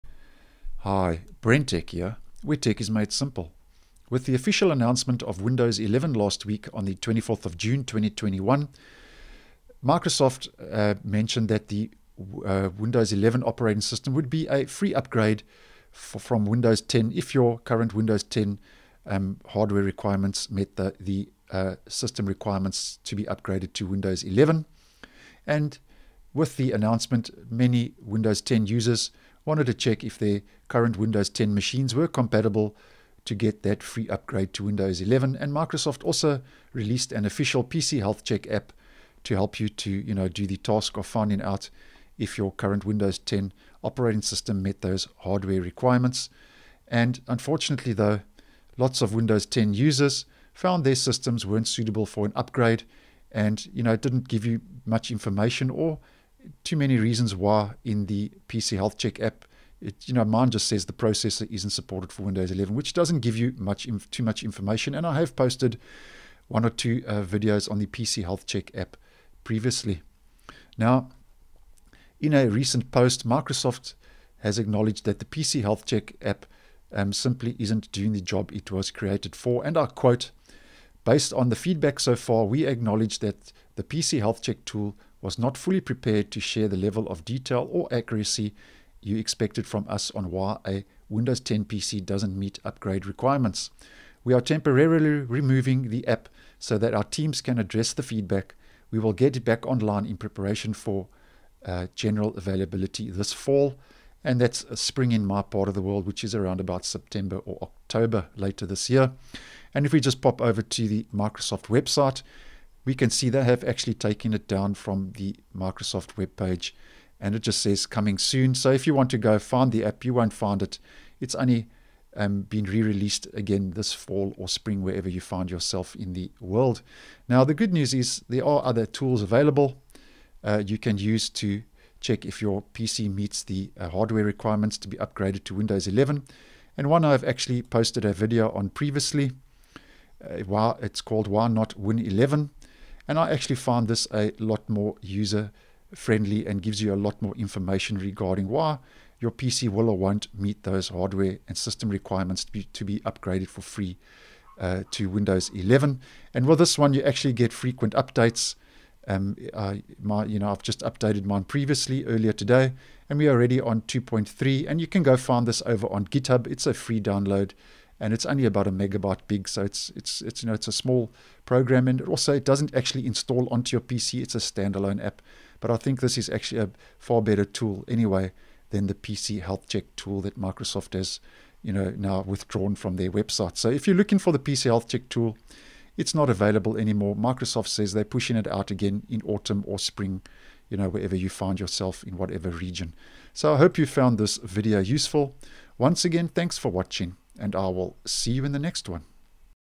Microsoft removes its PC Health Check Windows 11 upgrade app
Microsoft has acknowledged that the PC Health Check tool isn't doing the job it was created for.